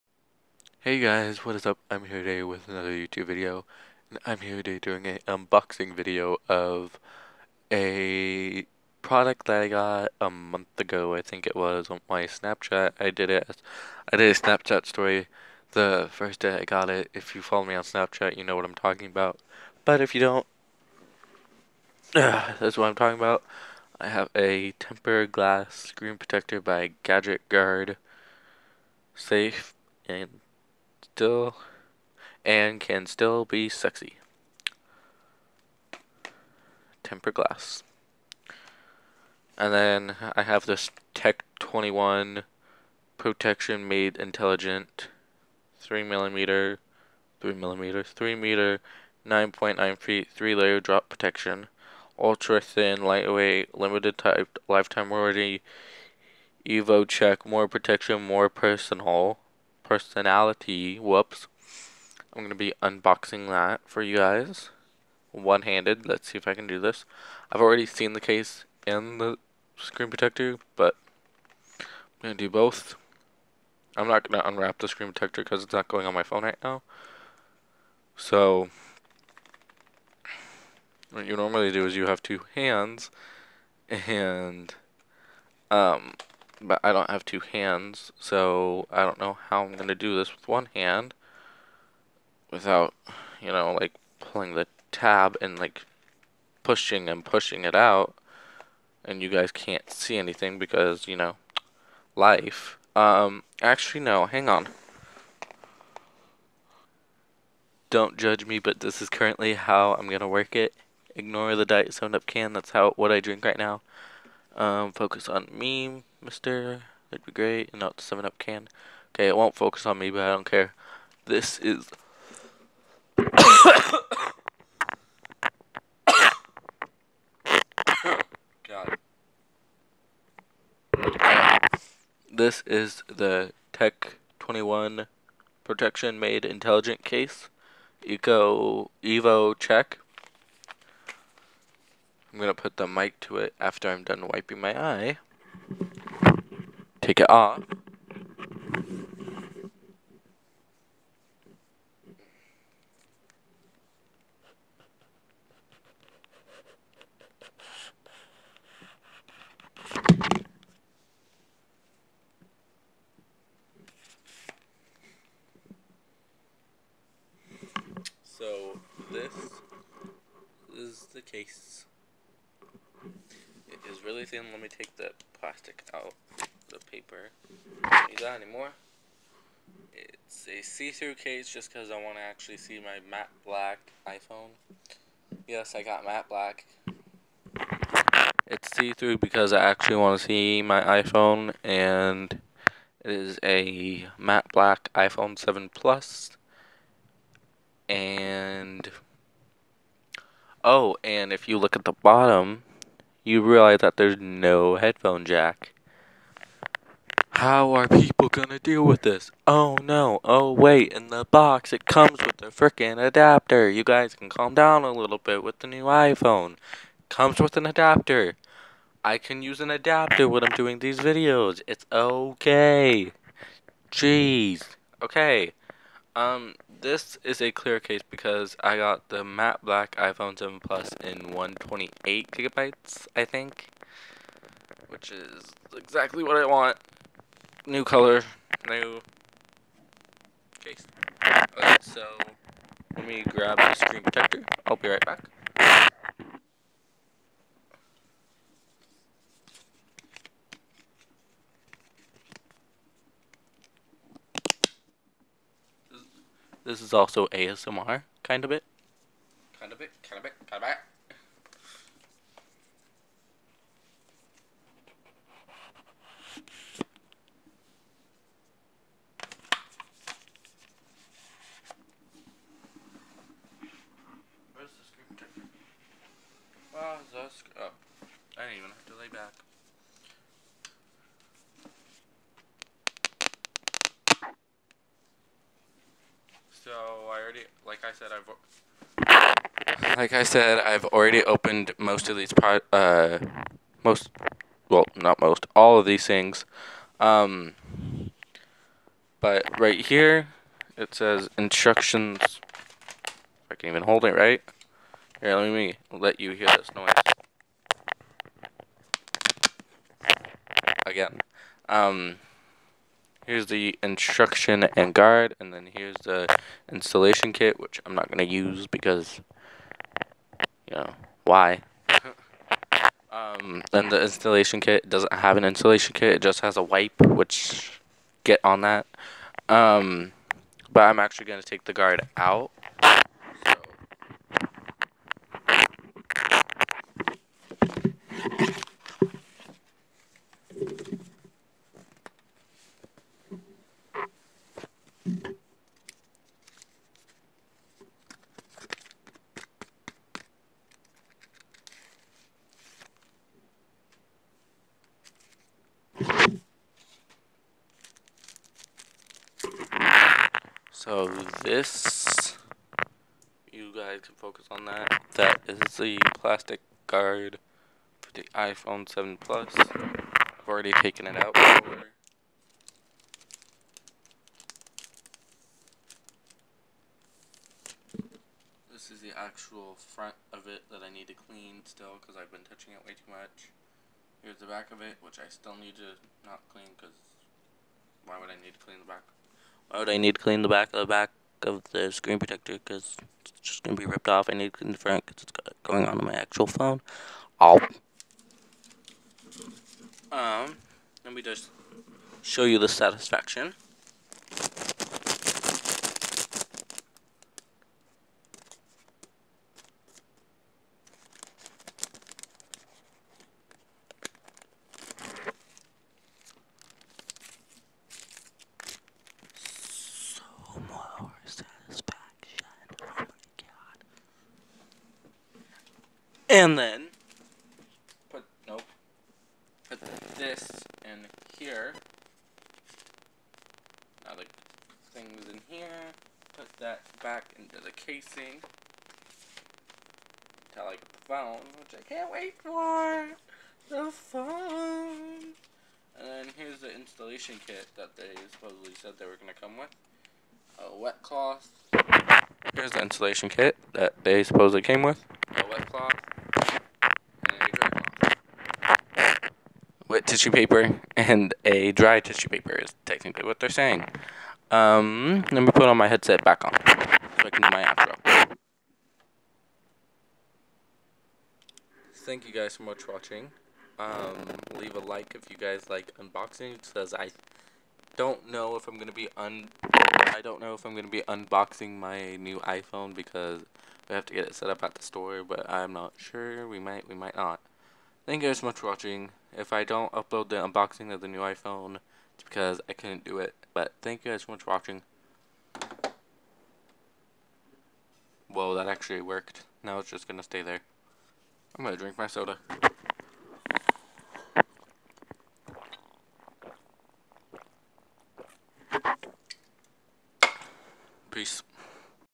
Kinda (ASMR UNBOXING) NEW iPhone 7 plus matte black case and screen protector
almost ASMR unboxing